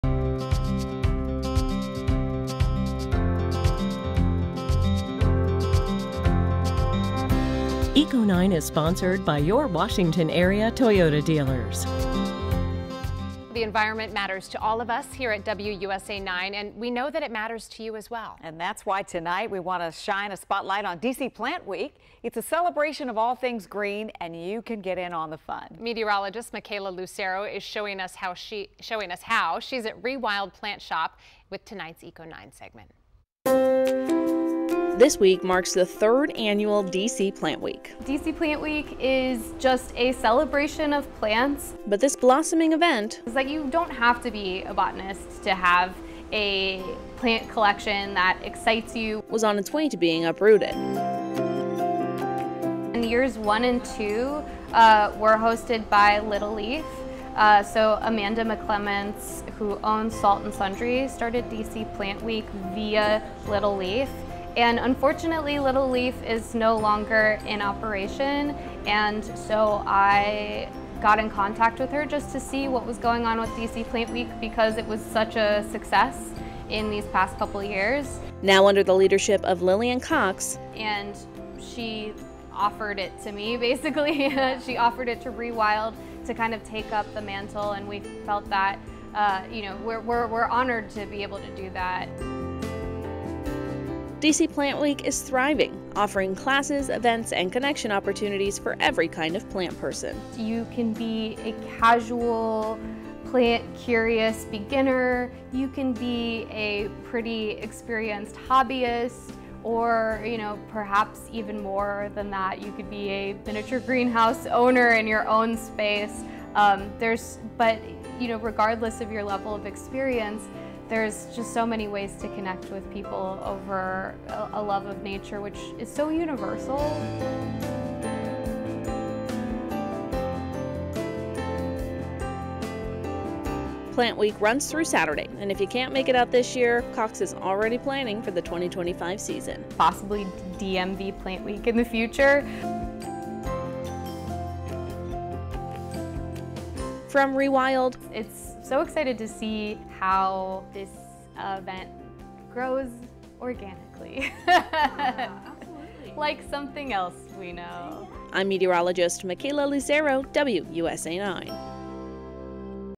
DC Plant Week aims to attract green thumbs, newbies alike to celebrate a love for nature
D.C. Plant Week is thriving, offering classes, events, and connection opportunities for every kind of plant person.  It runs through Saturday.